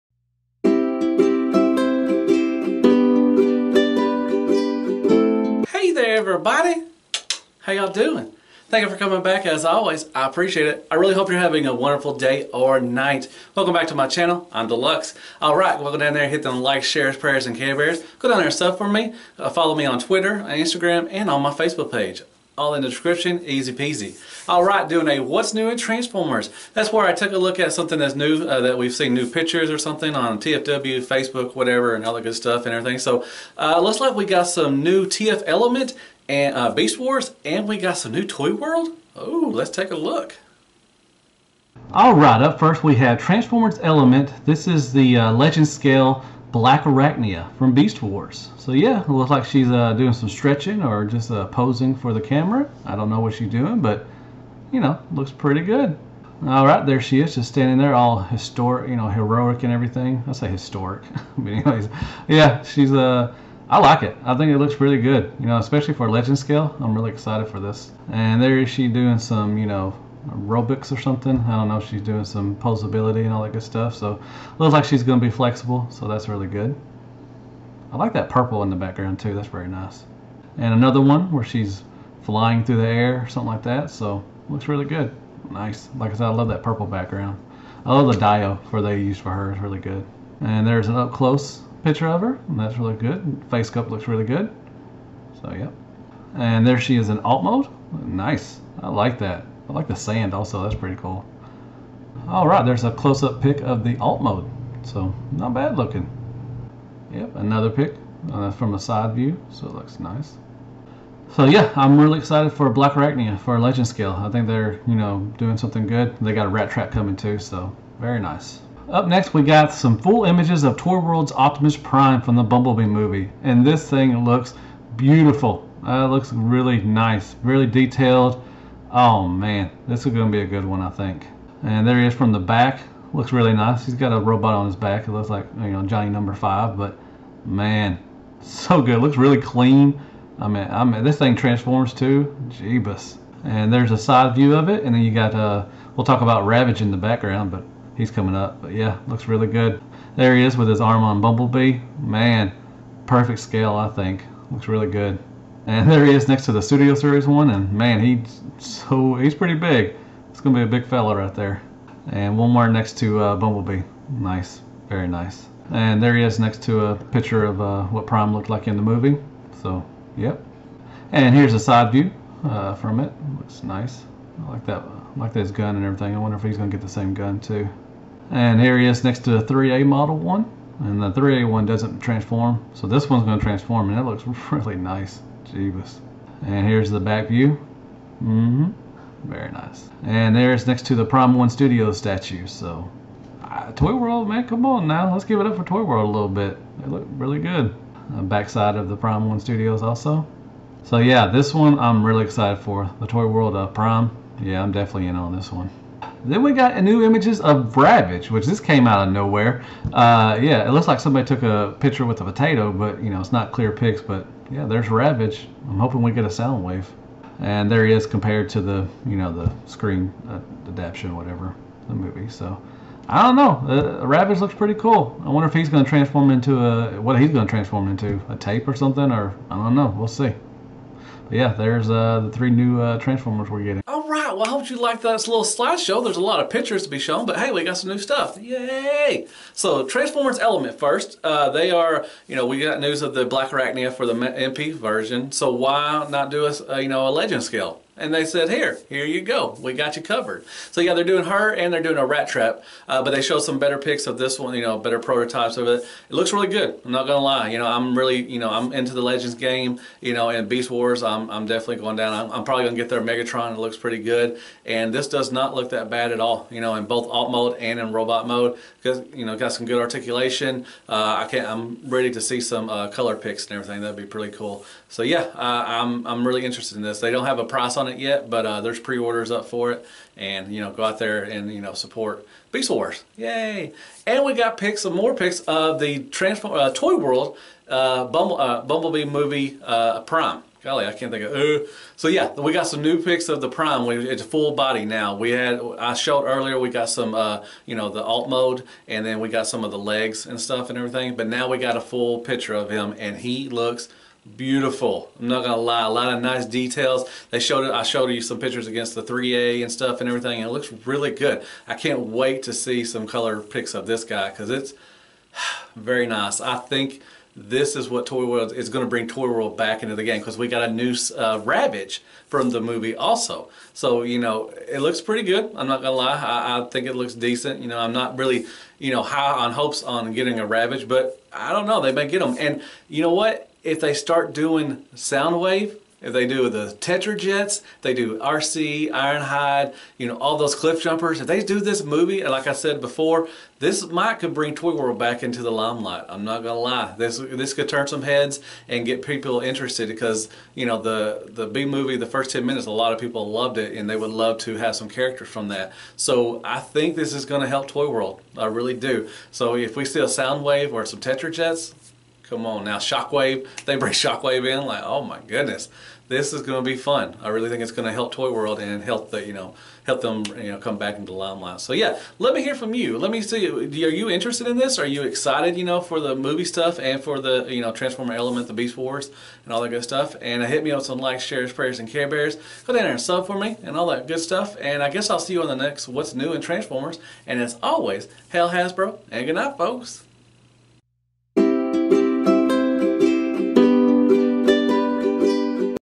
What's new in Transformers ? Up next 3rd Party TF Element blackarachnia and Toywold Prime and Ravage
What's new in Transformers? Up next 3rd Party TF Element (blackarachnia) and Toywolds (Prime and Ravage)!!
Check me out on Facebook, Twitter and Instagram down below